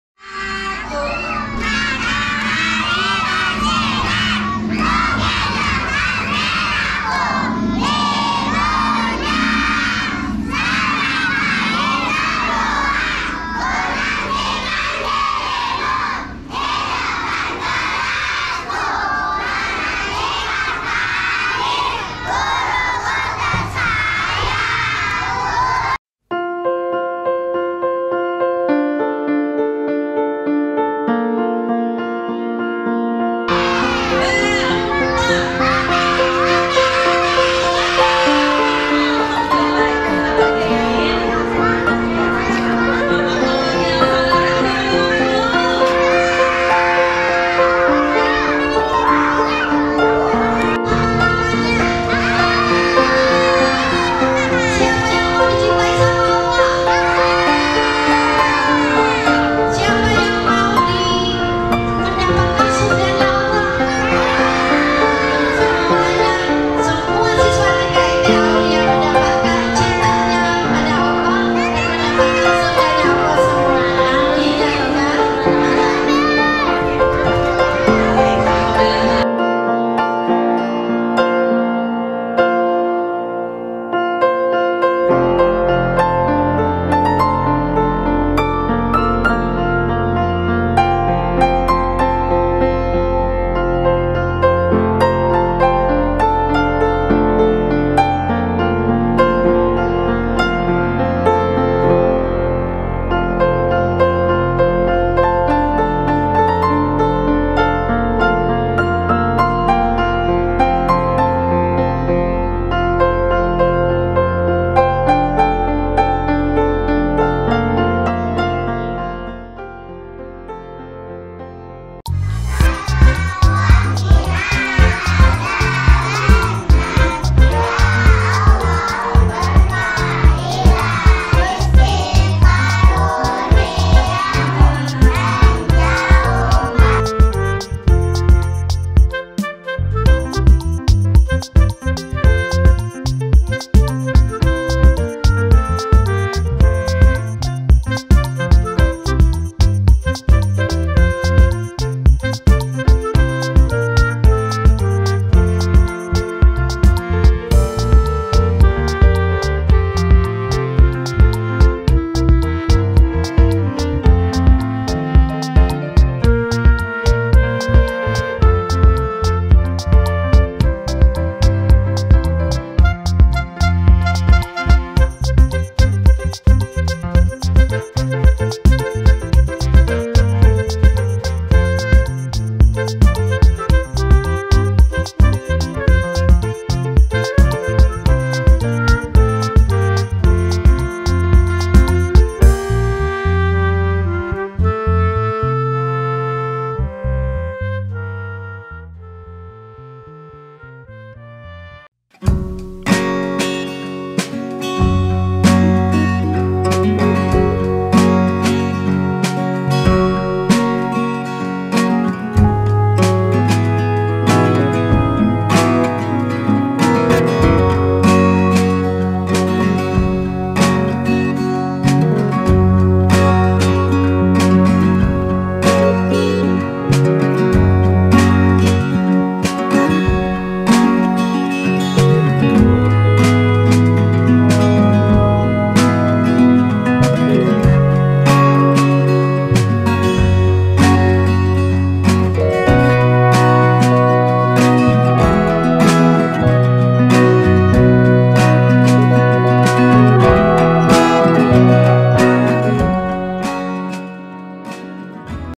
Yaya's Activity: Hari Guru di TKIT Aulia, Terima Kasih Bu Guru..
Pada tanggal 28 November lalu, Yaya dan semua teman di TKIT Aulia memperingati Hari Guru (yang seharusnya tanggal 25 November). Seperti halnya kasih sayang seorang Ibu, kasih sayang guru pun takkan mampu dibalas sampai akhir hayat.
semoga di momen ini, anak-anak paham bahwa selain Ibunya, gurulah yang menjadi perantara dari Allah ke mereka memberi ilmu untuk bekal di dunia dan akhirat. Sehingga mereka mampu menghargai, menyayangi dan patuh pada guru mereka.
terima kasih bu guru atas semua ilmu, pengabdian, kasih sayang untuk kami.
semoga Allah balas dengan beribu keberkahan. Aamiin ya robbal'alamiin...